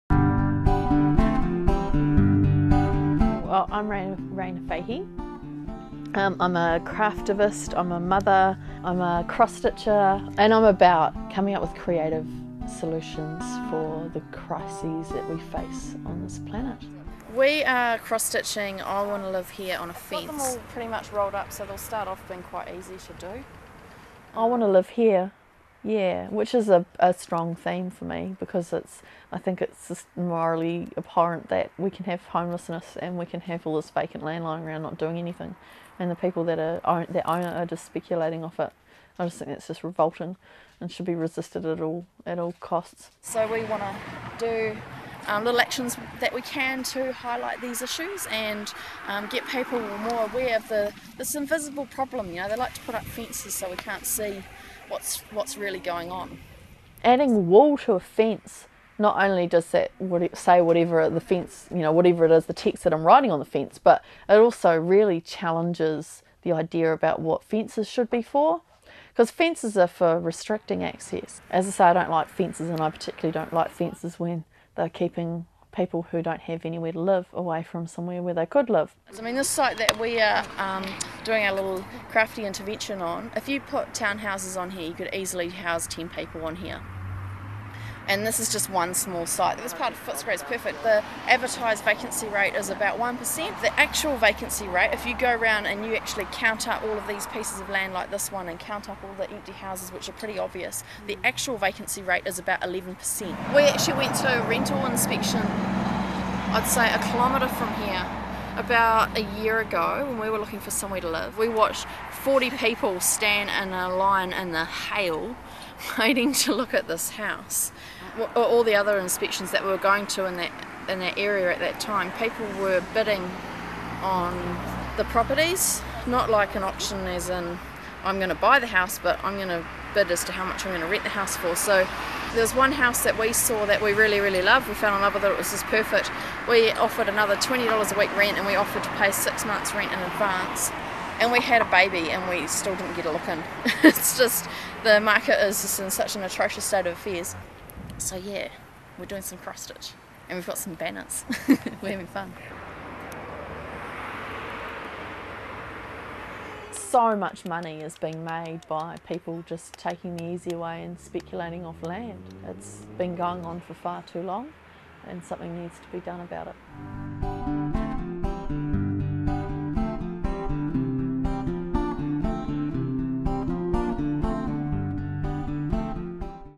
I Wanna Live Here
Director Anna Brownfeld is the winner of the first I Want to Live Here film competition. She interviews Rayna Fahey of Radical Cross Stitch on her fence stitching of vacant land. Why does this trend of vacant land exist during Australia's worst ever housing crisis?